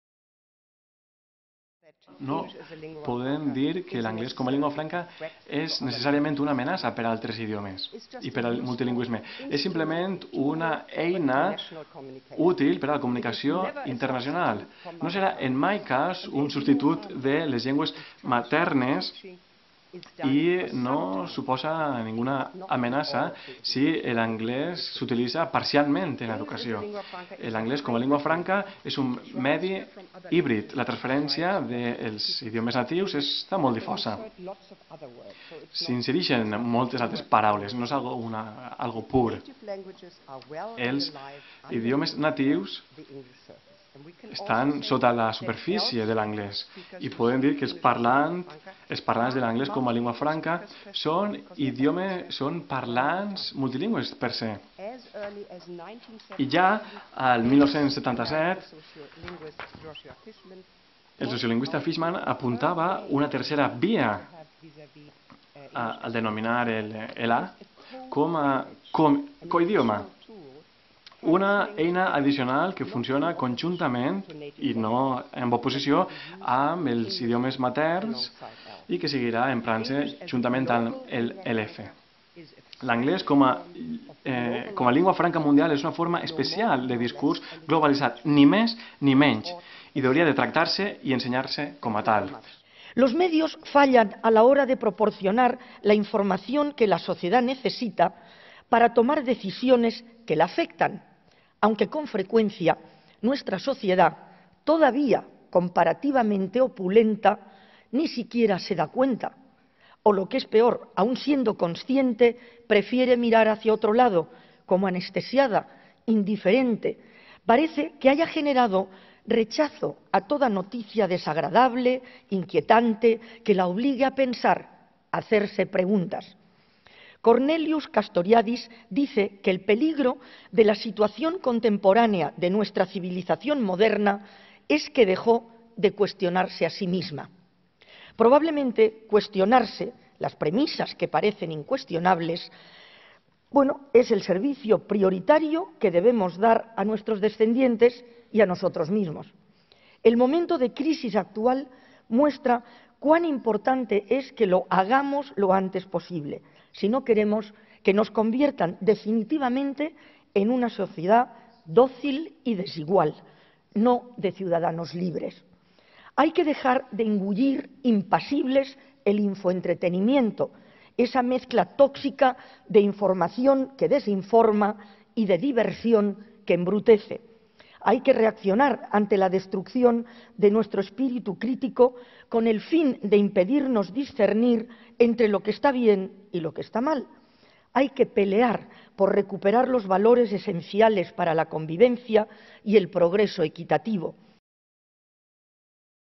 L'UJI nomena doctores honoris causa la professora Juliane House i la periodista Rosa María Calaf
La Universitat Jaume I ha investit com a doctores honoris causa Juliane House, professora de la Universitat d'Hamburg i un referent en la investigació en lingüística, i Rosa María Calaf, periodista reconeguda per la seua trajectòria humana, acadèmica i professional en l'àmbit del feminisme, la igualtat i el periodisme compromès. L'acte ha sigut presidit pel rector, Vicent Climent; el president del Consell Social de l'UJI, José Luis Breva, i el director general d'Universitat, Estudis Superiors i Ciència, José Miguel Saval.